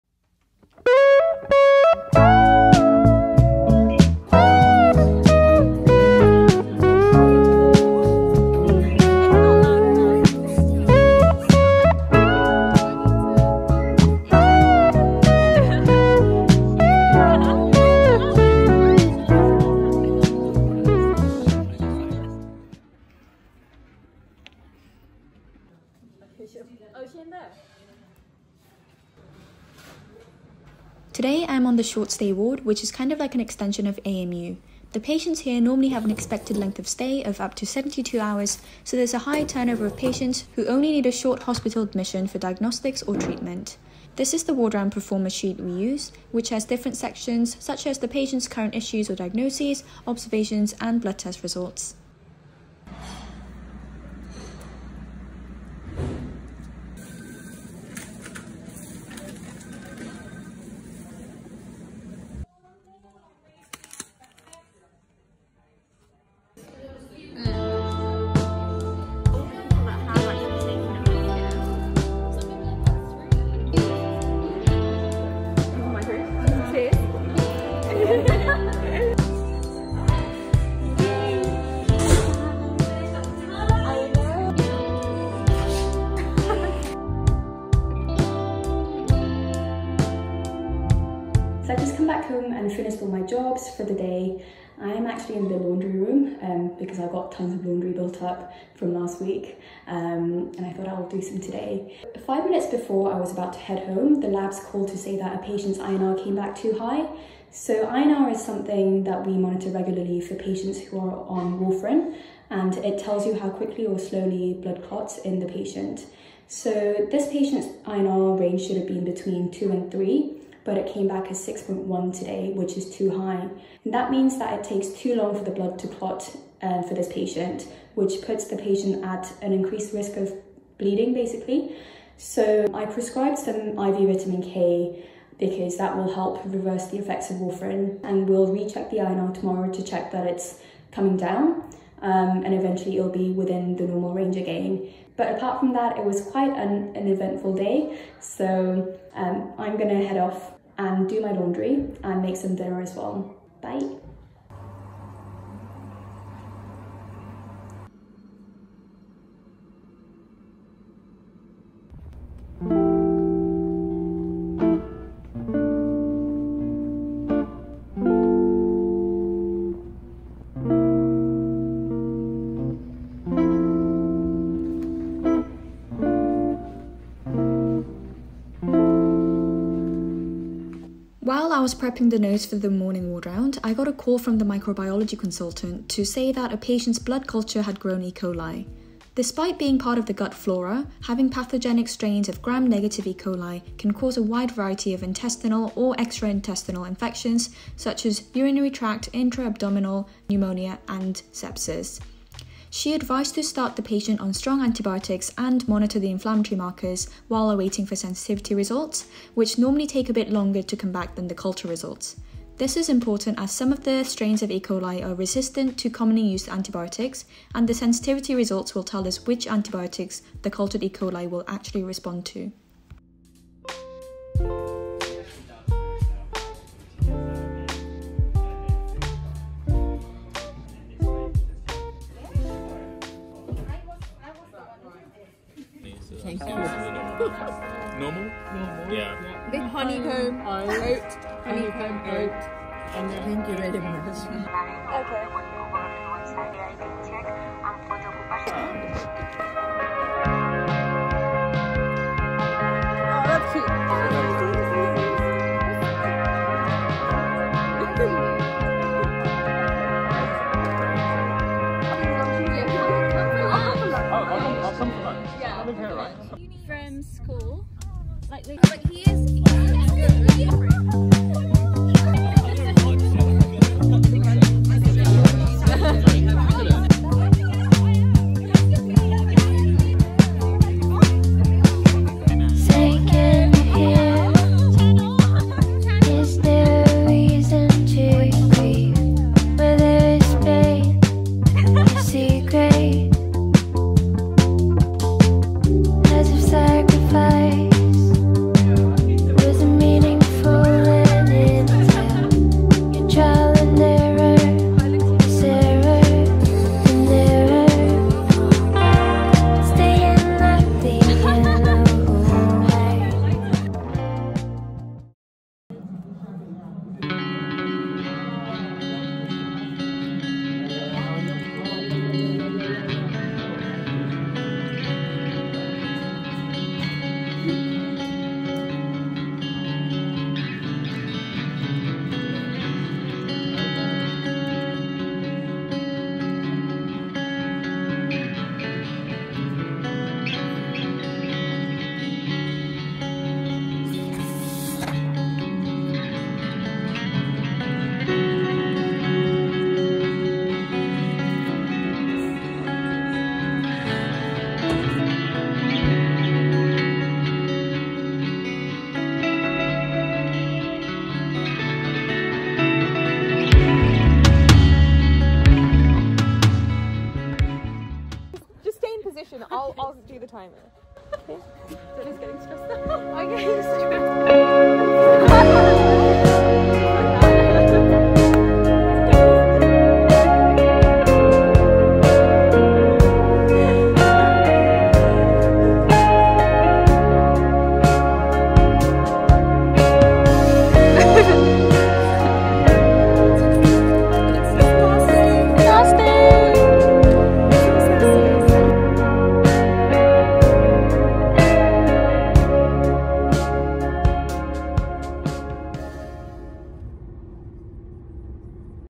junior doctor diaries | last week of fy1!
editing this video brought back so many memories of fy1 - i can’t believe i am a second year junior doctor now! we had an end of fy1 party to celebrate all the hard work we’ve put in over the past year, and it was so great to see everyone together before we all went off to different hospitals and started our jobs as brand new fy2s. thank you all for being here xx